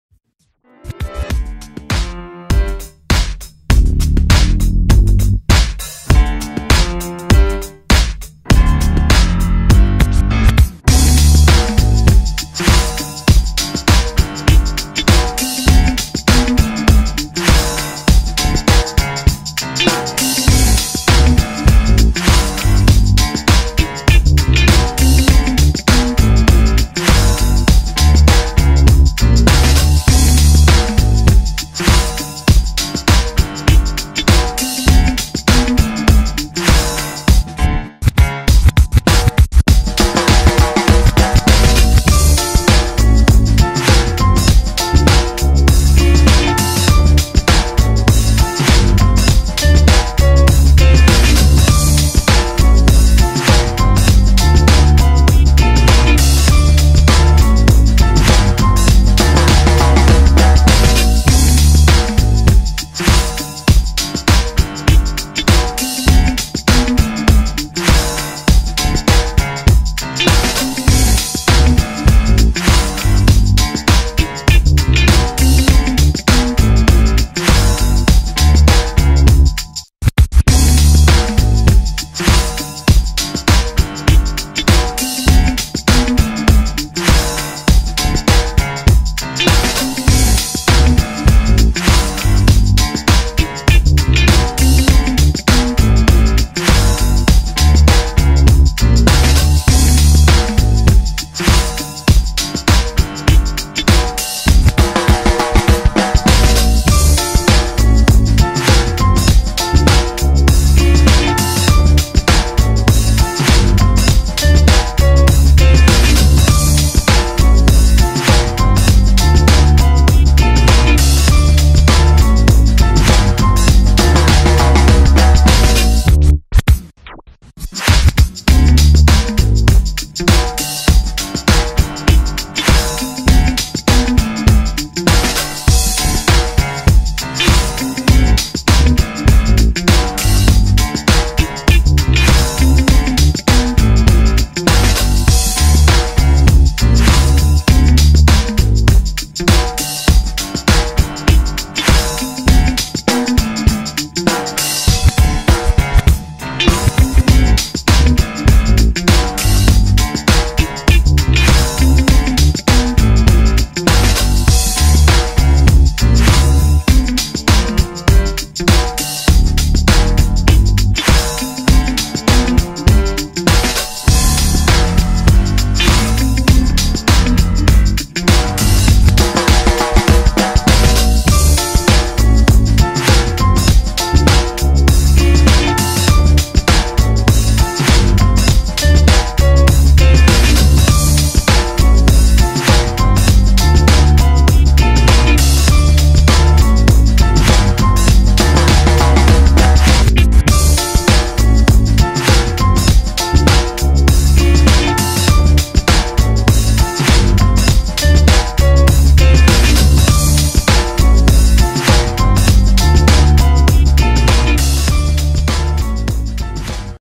Steampunk Laser Pistol Test 1
Steampunk Laser Pistol;  Burn Tests 1 Video -Ladies and Gents, the  X12 mark 1